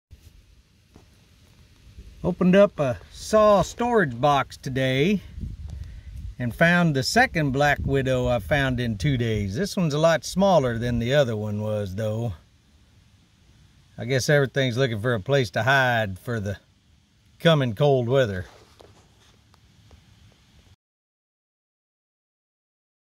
Northern Black Widow Spider
Northern Black Widow in a chainsaw box.  This is the second Black Widow I've found in two days.  The first was a much larger Southern Black Widow that was in a well house I was working on.